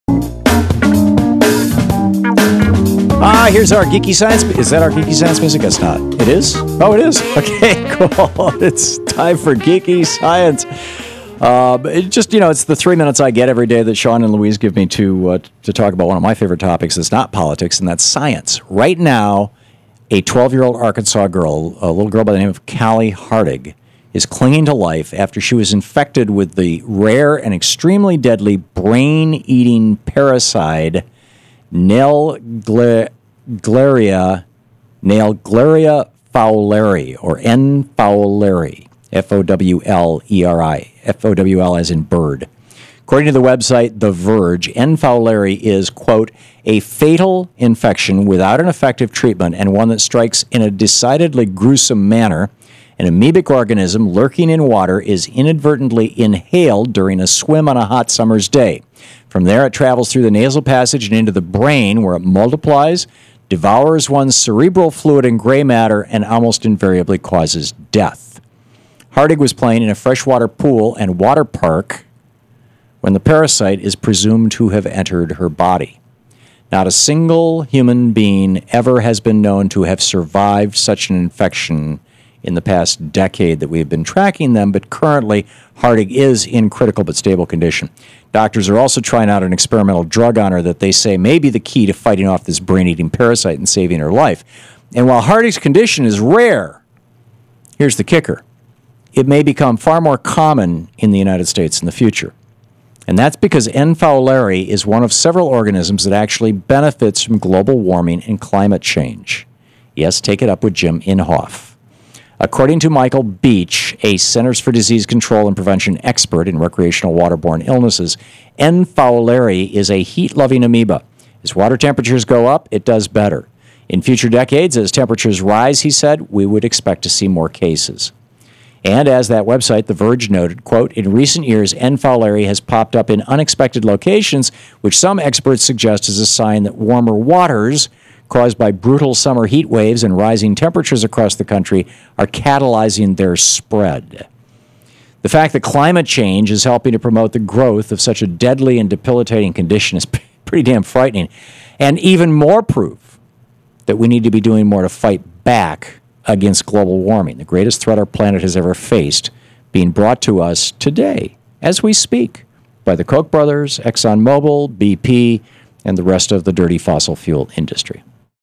Climate Change and Brain-Eating Parasites
In his Geeky Science segment, Thom Hartmann shares how a rare brain-eating parasite is thriving in warmer than usual waters.